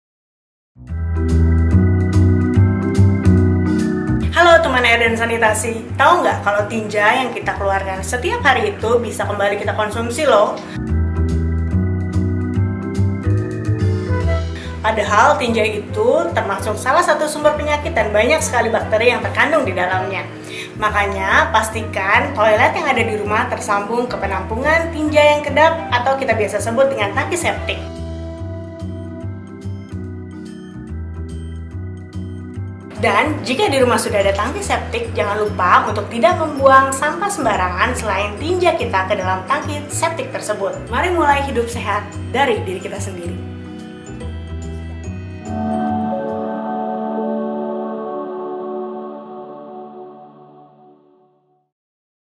Sudahkah rumahmu punya tangki septik kedap?
Halo TemanAirSanitasi!
TahukahKamu kalau tinja yang kita keluarkan setiap harinya bisa kembali kita konsumsi loh! Faktanya tinja yang tidak tertampung di tangki septik dapat mencemarkan sumber air dan makanan yang dikonsumsi.